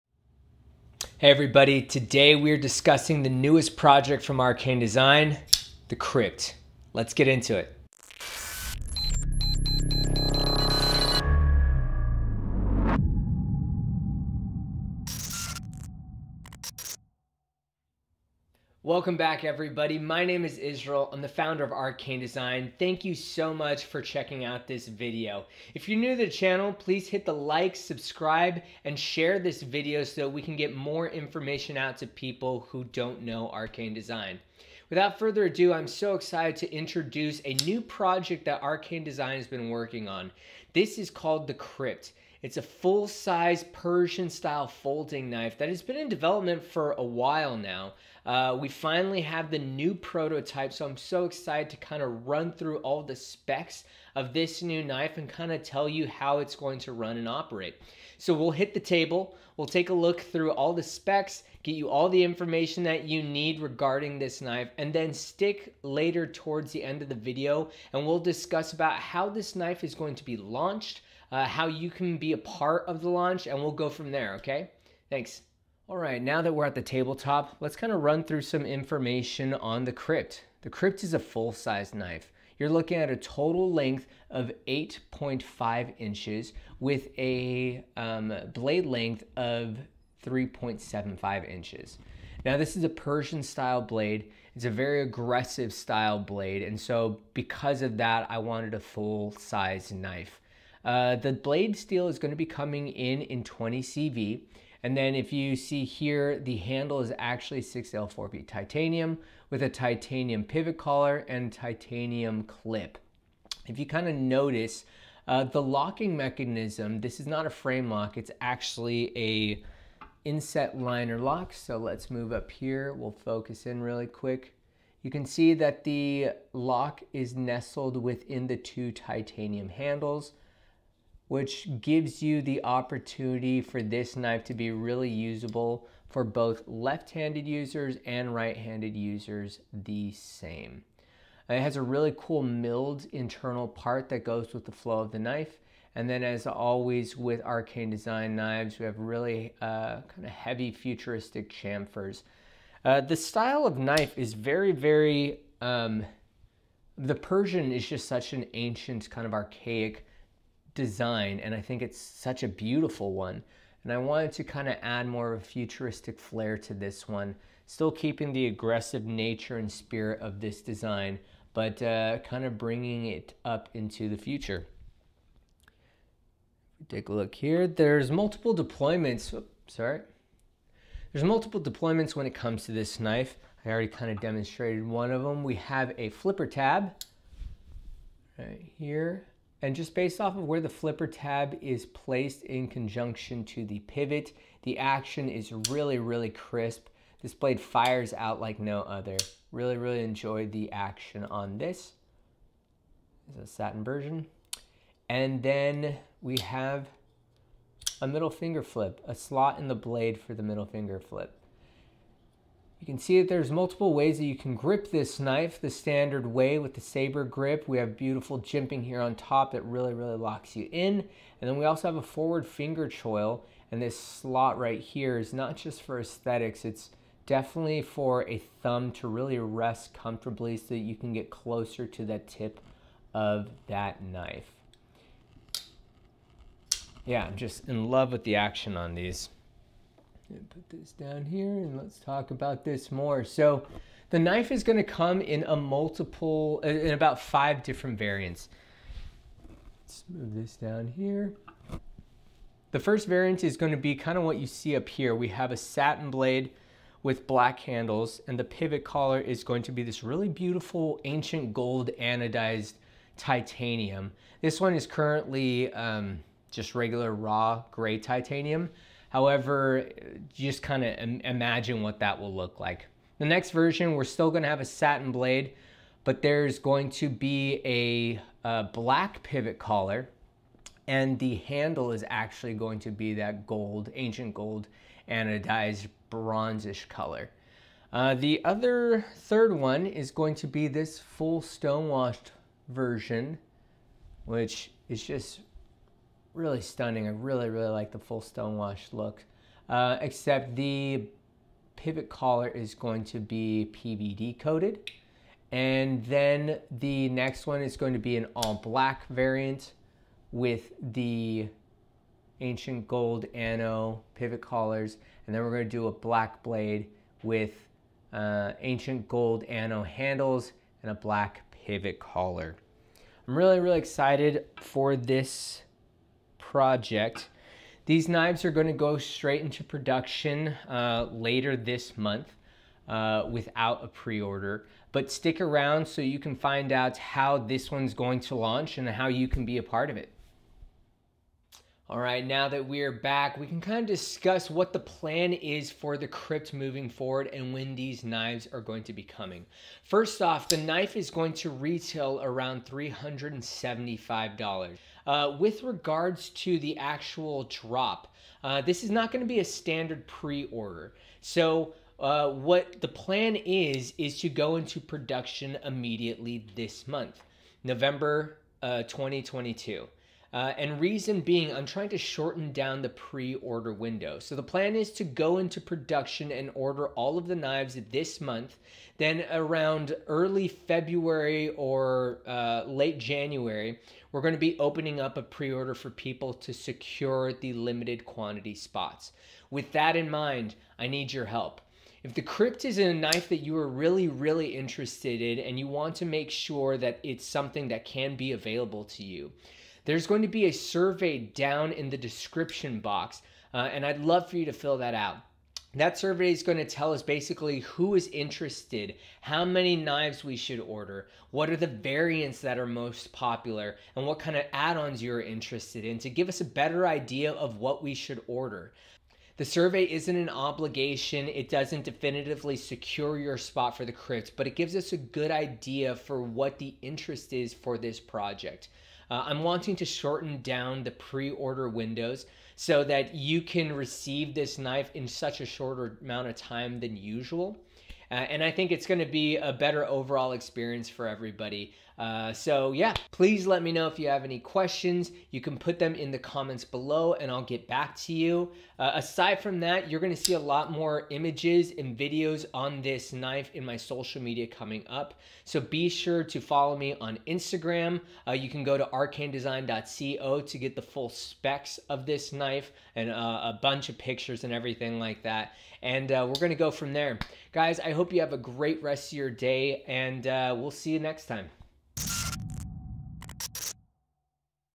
THE CRYPT by Arcane Design
THE CRYPT is the latest product of Arcane Design. The pre-order for this knife is coming 2023. In the mean time, If you are interested in purchasing this knife, Please click the URL below and fill out the interest survey: